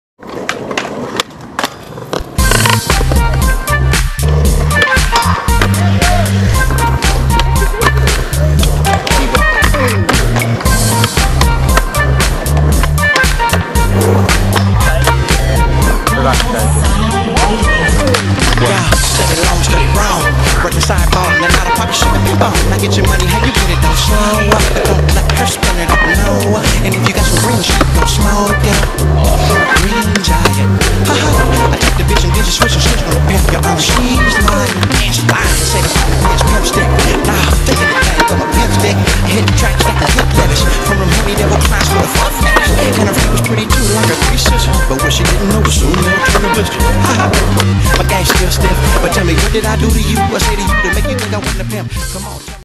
shreding the gnar at legacy.
shreding the legacy rail.
Ryan Racine
John Pie
Noah Cleroux
Tony Giraldo
Others